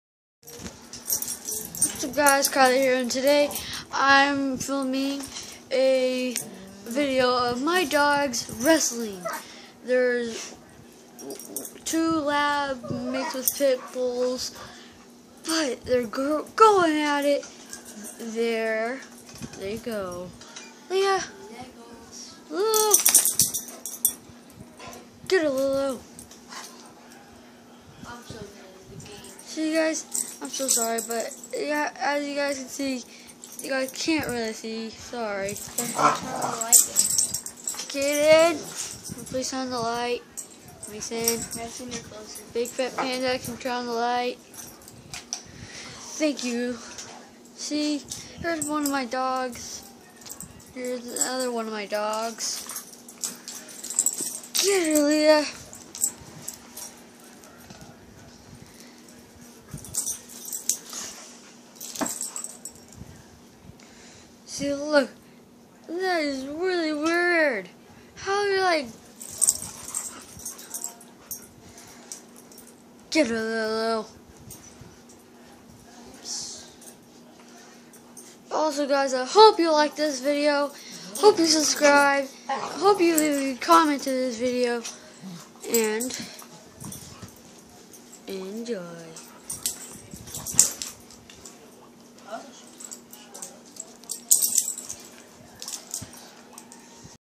my two dogs wrestling in my bedroom
My two dogs wrestling in bedroom I just wanted to show you guys